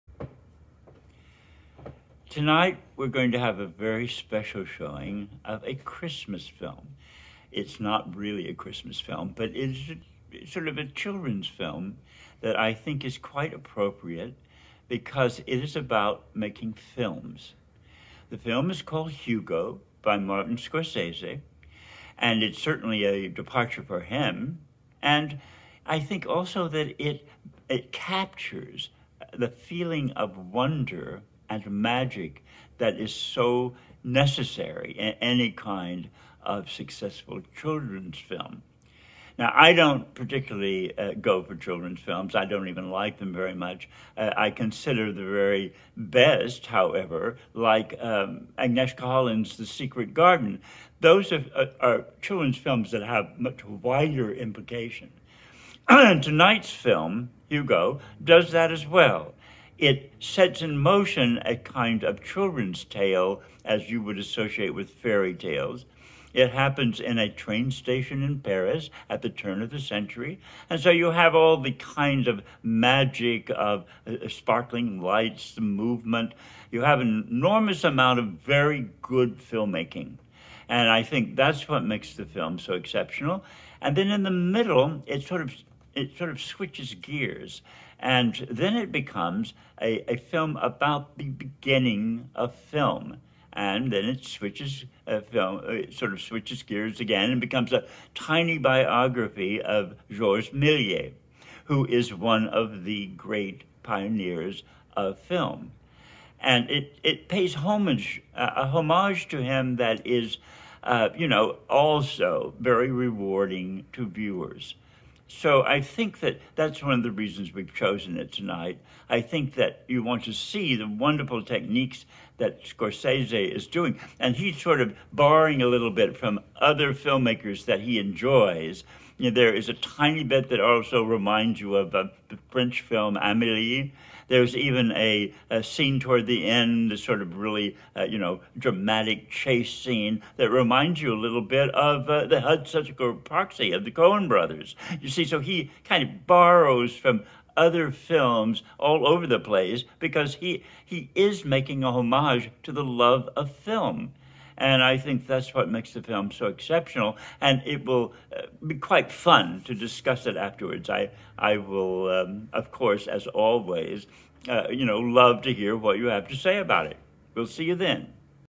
Ronald Chase's intro to Scorsese's Hugo (2011)
For SF Art & Film's Cine Club